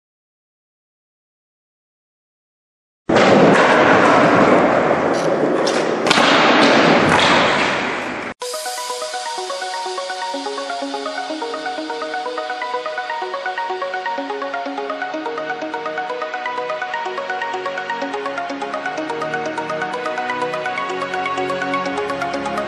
The New Trick #2 : Nollie Varial Flip
I was close of landing it fo too long haha and now i finally have it :)